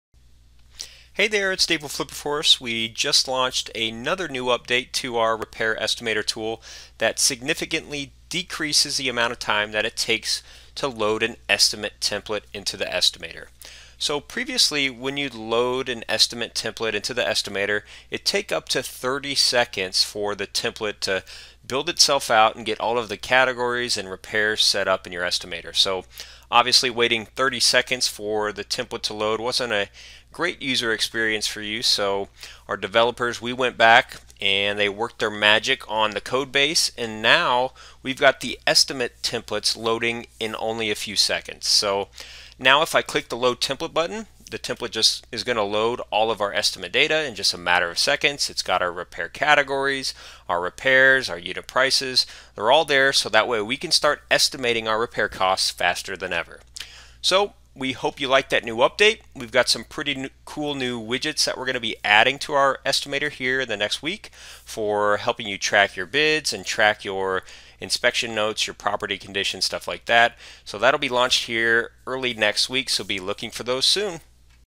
Estimator Templates Loading Blazing Fast 🔥
We just launched another new update to our Repair Estimator tool that significantly decreases the amount of time it take to load an estimate template into the estimator.